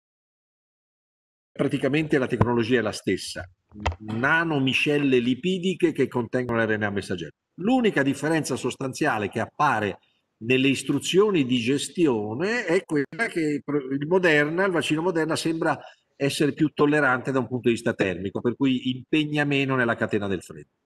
Covid-19 e vaccino, la parola all'esperto 6
Il professor Giovanni Di Perri, ordinario di malattie infettive e primario all'ospedale Amedeo di Savoia di Torino, risponde alla domanda: quale differenza esiste fra il vaccino di Pfizer e quello di Moderna?